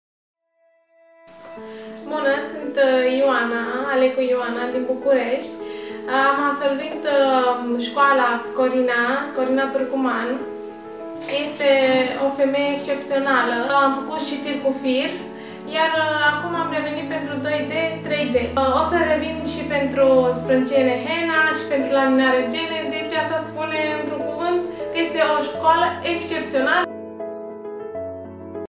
Curs Extensii Gene Metoda 2D 3D Impresii
Curs Extensii Gene Metoda 2D 3D.
Ioana absolventa Cursului de baza, adecis sa-și răsfețe clientele cu genuțe de volum, absolvind Curs Extensii Gene Metoda 2D3D organizat de Studio-Salon KARINA LASHMAKER